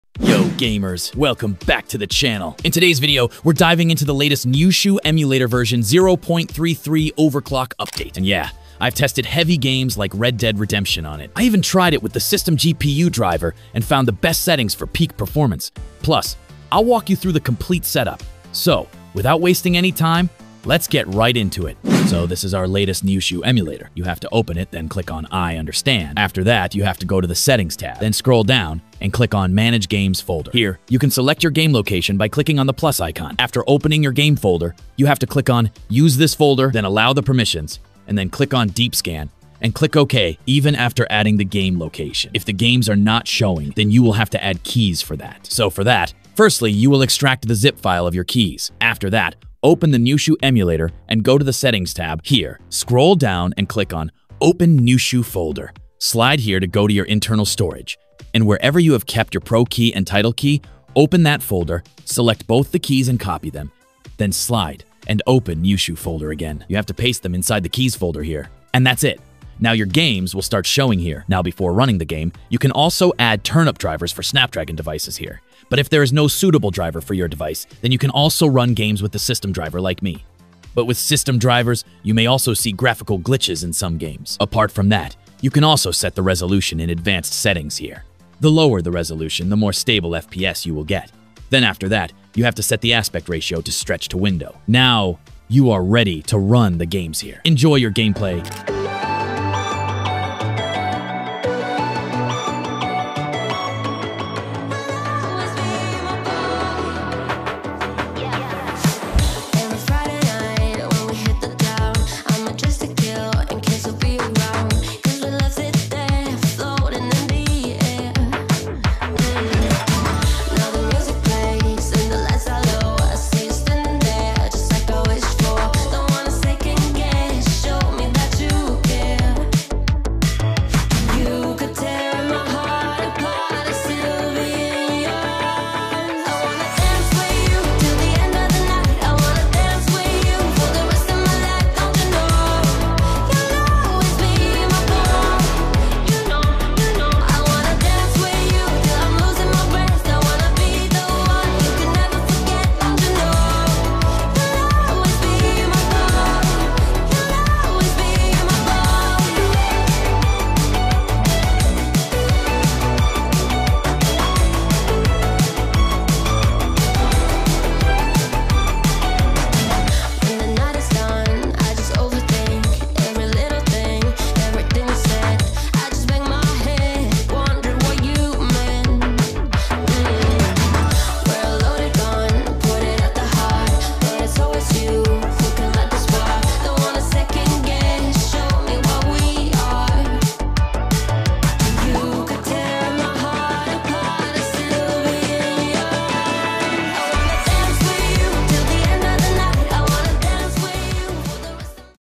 🔥 NYUSHU Emulator Android V33 - Full Setup | System GPU Driver - Nyushu Switch Emulator Overclock
🔥 The latest NYUSHU Emulator Android V33 update is here! 🚀 This update brings major improvements with System GPU Driver support and enhanced overclocking options for smoother performance. In this video, I’ll guide you through the complete setup and best settings for maximum FPS and improved stability on the Nyushu Switch Emulator! 🎮

💥 Key Highlights:
✅ Nyushu Emulator V33 Update Overview
✅ System GPU Driver for Improved Performance
✅ Overclocking for Higher FPS
✅ Best Settings for Stable Gameplay
✅ Full Setup Guide & Fixing Common Issues

📱 Test Device Specifications:
✅ Device: Realme GT 7 Pro
✅ Processor: Snapdragon 8 Elite
✅ RAM: 16GB